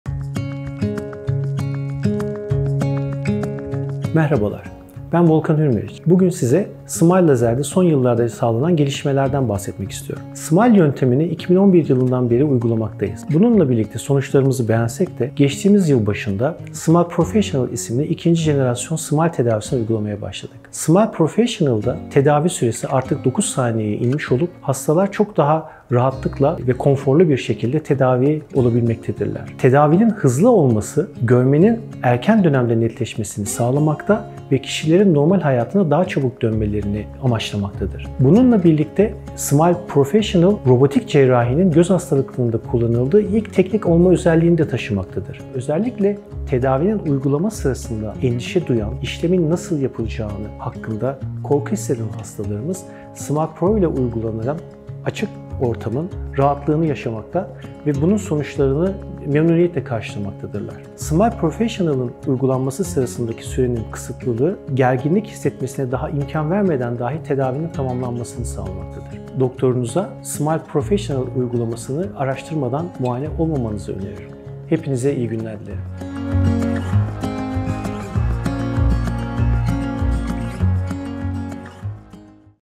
Smile Pro Lazer Tedavisi Nedir? | Doç. Dr. Volkan Hürmeriç Açıklıyor
Smile Pro Lazer Tedavisi miyopi ve astigmatizm gibi refraktif kusurları düzeltmek için uygulanan lazer göz ameliyatı yöntemidir.

Peki Smile Pro lazer tedavisi nedir? Nasıl yapılır? Kimler için uygundur? Bu videomuzda, Doç. Dr. Volkan Hürmeriç, Smile Pro lazer tedavisi hakkında bilmeniz gereken tüm bilgileri açıklıyor.

Smile Pro lazer tedavisi nedir?
Smile Pro lazer tedavisi nasıl yapılır?
Smile Pro lazer tedavisi kimler için uygundur?
Smile Pro lazer tedavisinden sonra nelere dikkat edilmelidir?

Tüm bu soruların cevabı için videomuzu izleyebilirsiniz.

❕Sayfa içeriği bilgilendirme amaçlıdır.

Doktorumuzun Bilgileri İçin Aşağıdaki Linke Tıklayabilirsiniz.